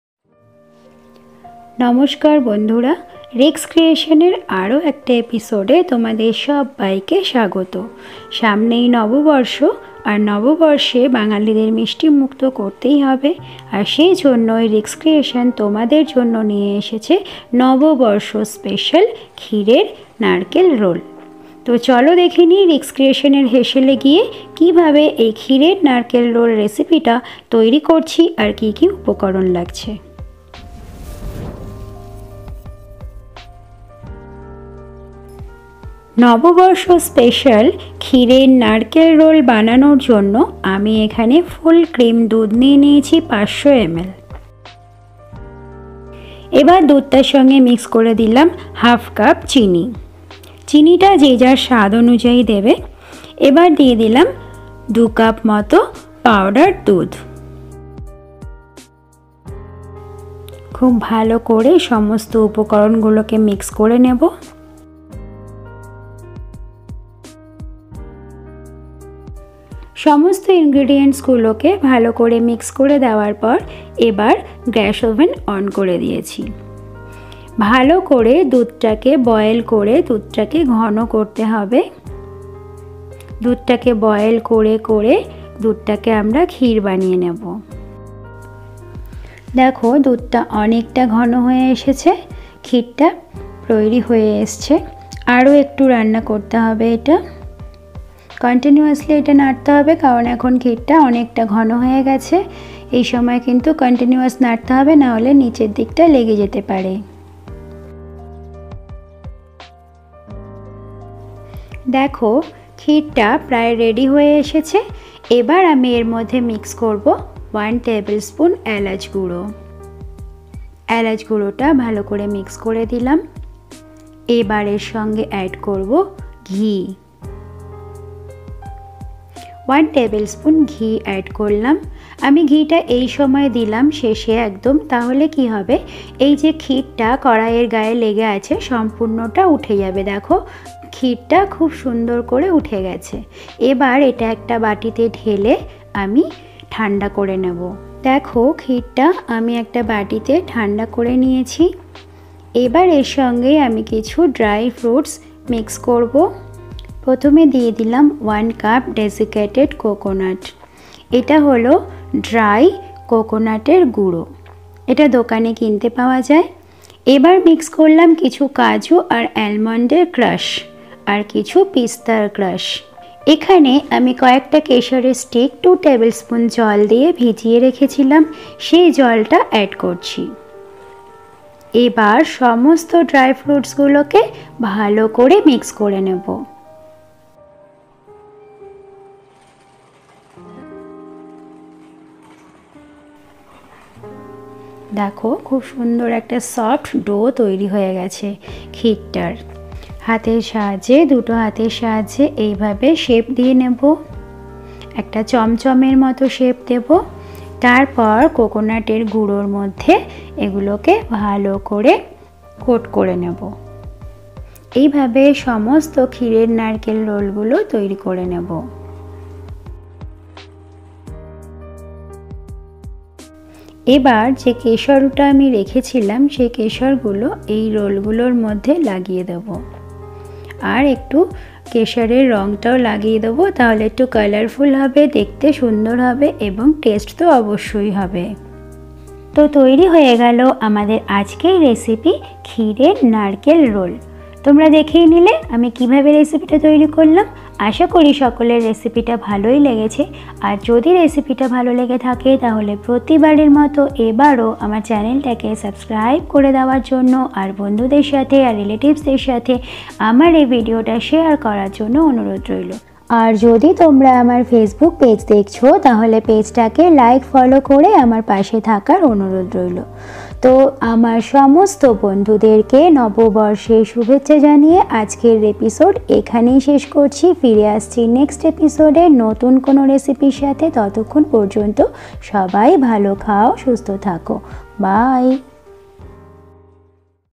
নববর্ষ স্পেশাল ক্ষ্মীরের নারকেল রোল রেসিপি | Malai Coconut Rolls Recipe in Bengali | Somar Rannaghar
সবাইকে শুভ নবর্ষের শুভেচ্ছা জানিয়ে আজকের ভিডিও শুরু করছি।
আজ আমি তোমাদের ক্ষ্মীরের নারকেল রোল তৈরি করে দেখাবো যেটা তোমরা এই নববর্ষ তে ঘরেই তৈরি করে নিতে পারো।

♨️ Ingredients :-
(1). 500 ml. Full Cream Milk
(2). ½ cup. Sugar
(3). 2 cup. Powder milk
(4). 1 tbsp. Cardamom Powder
(7). Cashew & Almond ( as required, crushed )
(8). Pistachio ( as required, crushed )
(9). Saffron ( as required, soaked in water )
(10). Desiccated Coconut ( for coating )